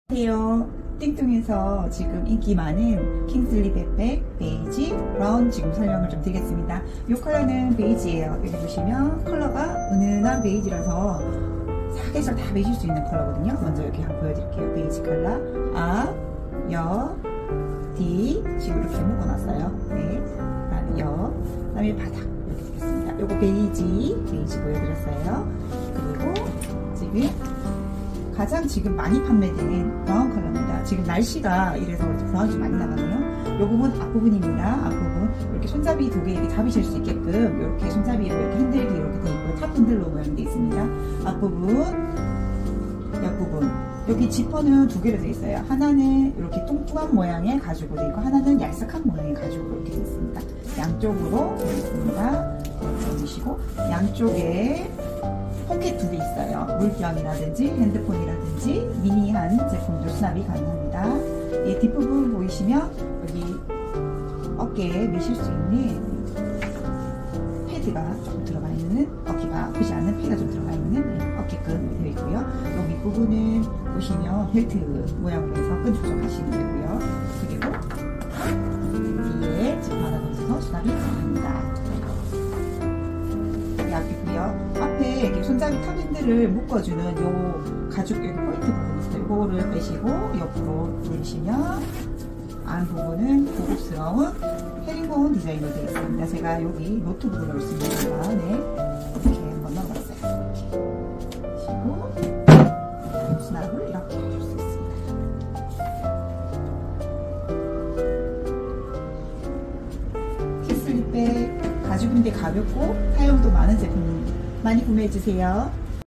[GLM] 띵뚱마켓 l 🔥 여성을 위한 천연 소가죽 킹슬리 백팩 BEST 👜
📌 주문 방법

재질: 고급스러운 천연 소가죽, 내구성 좋은 해링본 안감
특징: 노트북 수납 가능, 어깨 부담을 줄여주는 패드 스트랩, 다용도 외부 포켓 포함
컬러 옵션: 베이지, 브라운
추천 대상: 여성, 실용적이면서 스타일리시한 백팩을 찾는 분

✅ 세련된 디자인과 실용성을 겸비한 킹슬리 백팩, 지금 만나보세요!